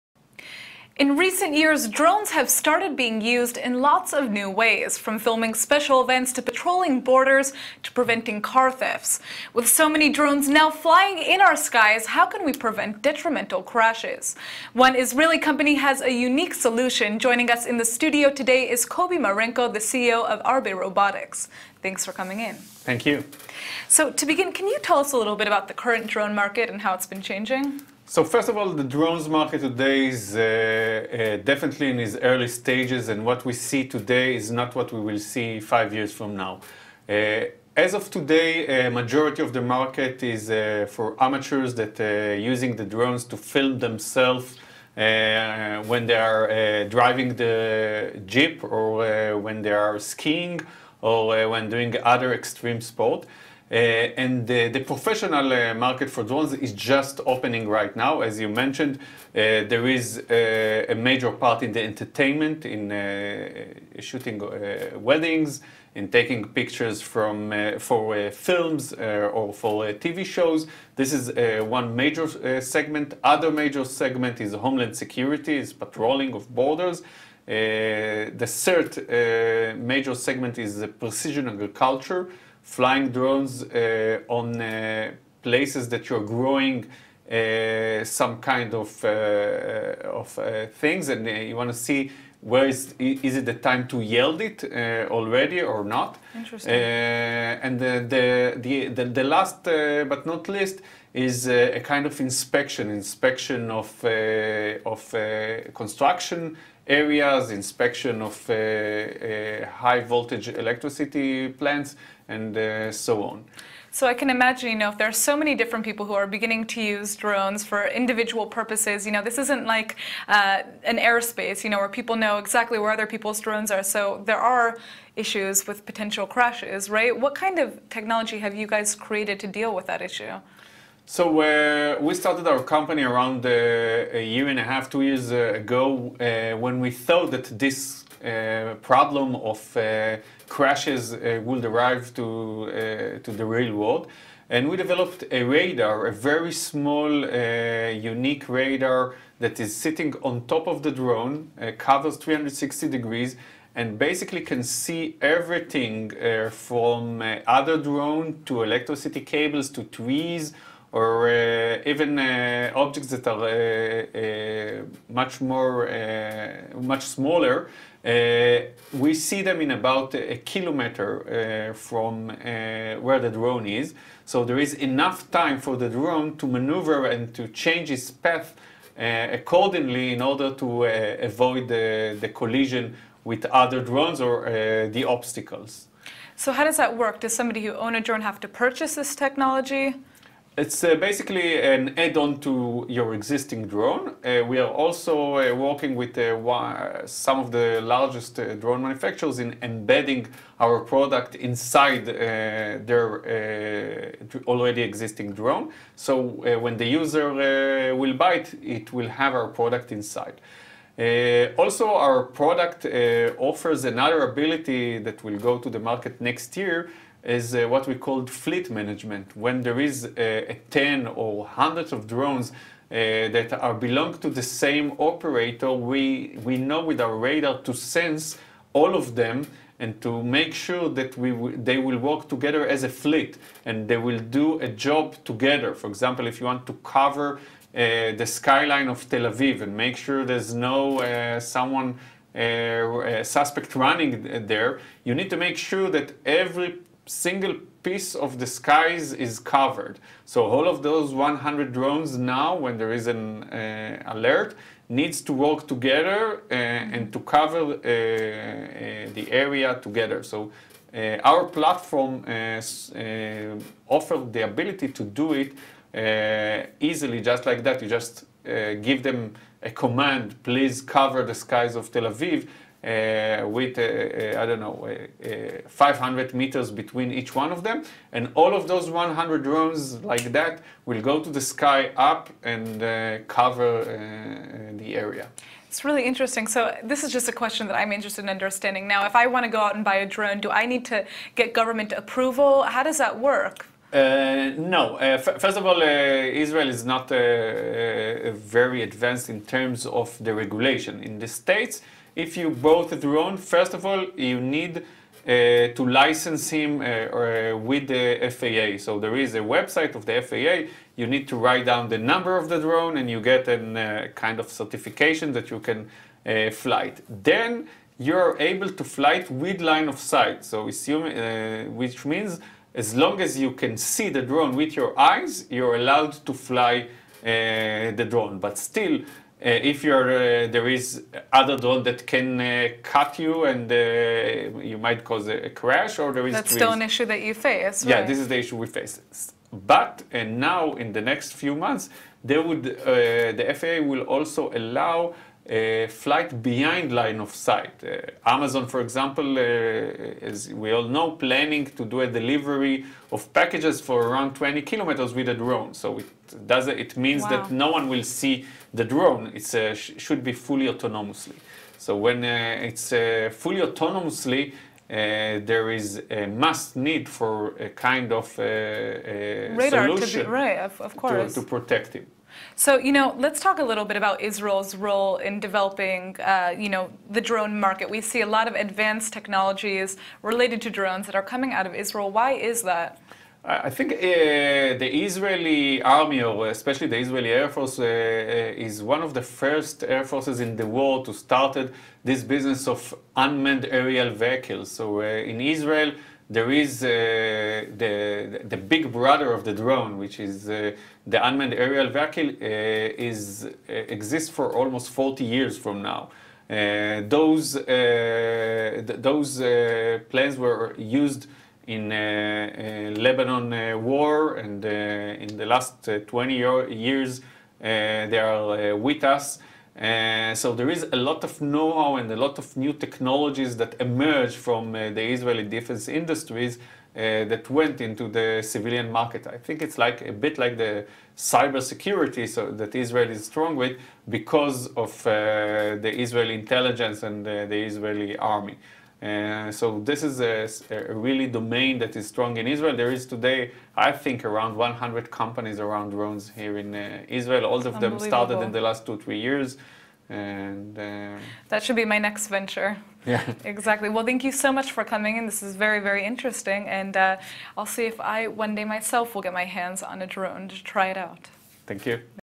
Kobi Marenko, CEO of Arbe Robotics - July 25, 2016
In recent years, drones have started being used in lots of new ways…. from filming special events to patrolling borders to preventing car thefts. With so many drones now flying in our skies...how can we prevent detrimental crashes? One Israel company has a unique solution. Joining us in the studio today is Kobi Marenko.